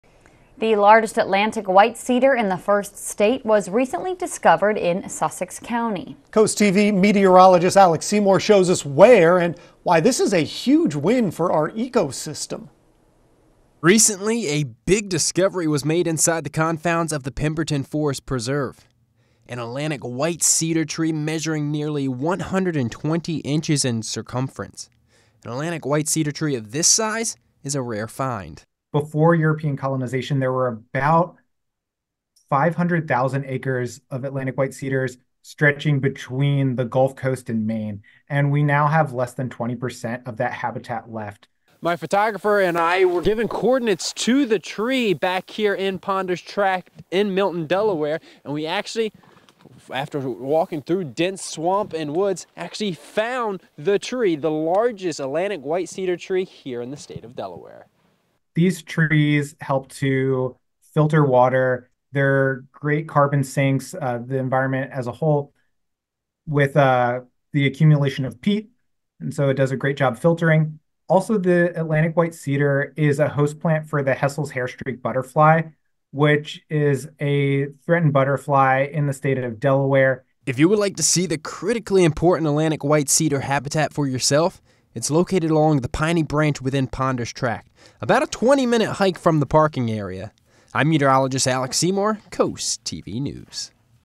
Delaware’s largest Atlantic White Cedar tree found in Sussex County
The Nature Conservancy recently announced the finding of the largest Atlantic White Cedar tree in the state of Delaware. The tree was found along the Piney Branch at Ponders Tract within the Pemberton Forest Preserve.